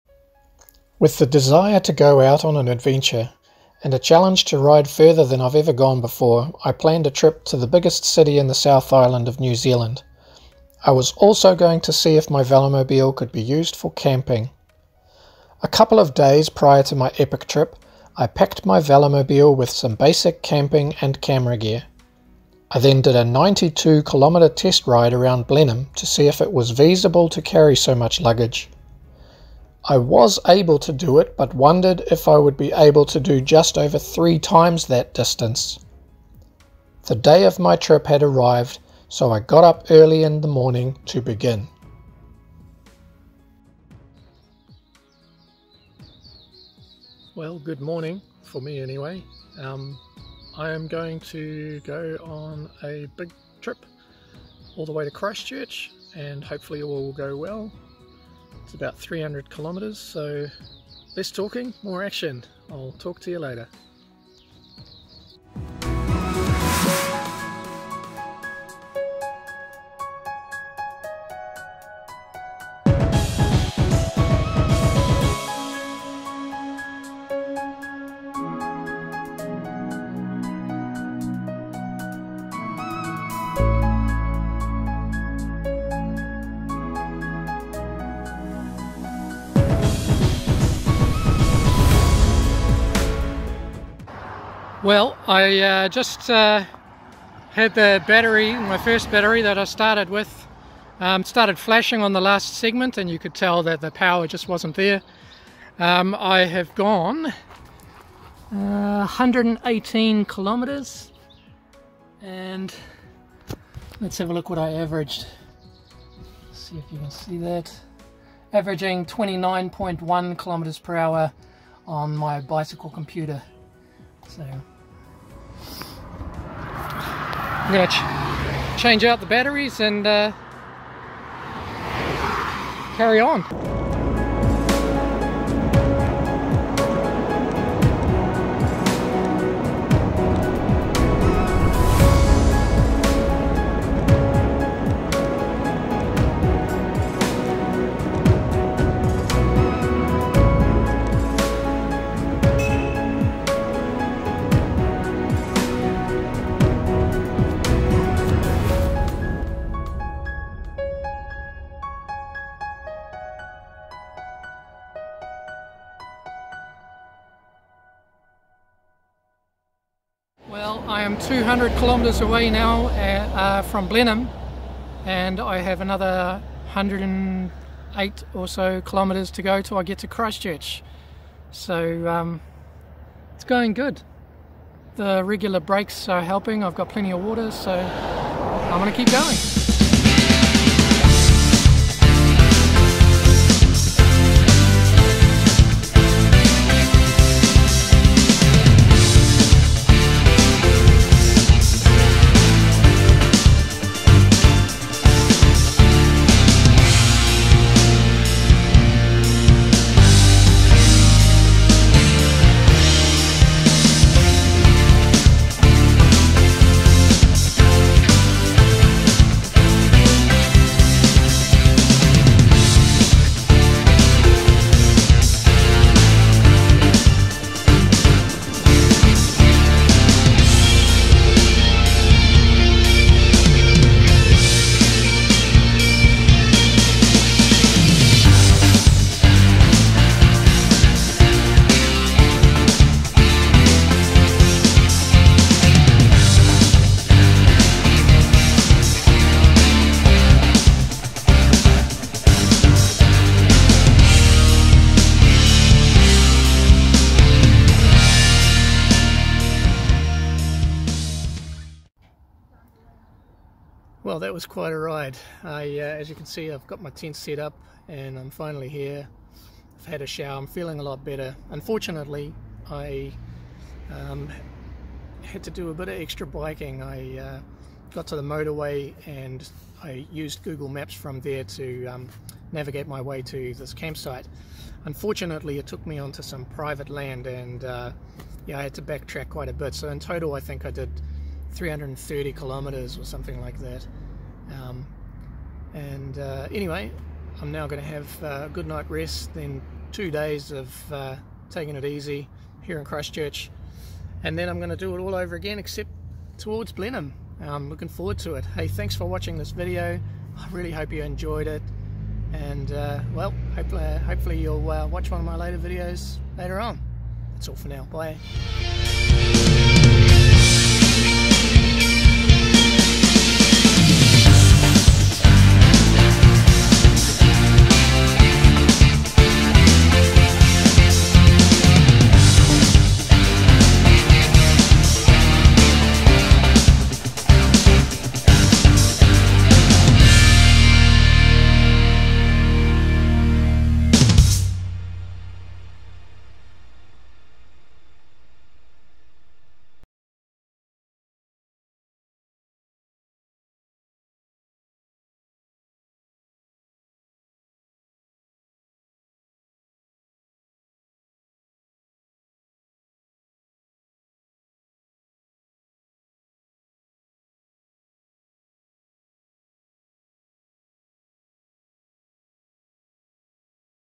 300km Electric Velomobile Camping Trip - Cycling to Christchurch and back with electric assist
Until this ride the most I had ever ridden in my velomobile was 257km.  This time I set out not only to ride even further, but also to take along a tent, sleeping bag, sleeping mat, sleeping bag liner, repair kit, cameras, tripod, camera mounts and food and water.
Assuming I would make it to my destination, Christchurch, I would have to bike back only two days later.  I had no idea whether I could really even do this.
I took my velomobile for a test run the day before, and it seemed like it would be possible.  So I packed my velomobile and set out.  This video shows a bit about my trip bike tour to Christchurch, I hope you enjoy it.

Marnix Mooiman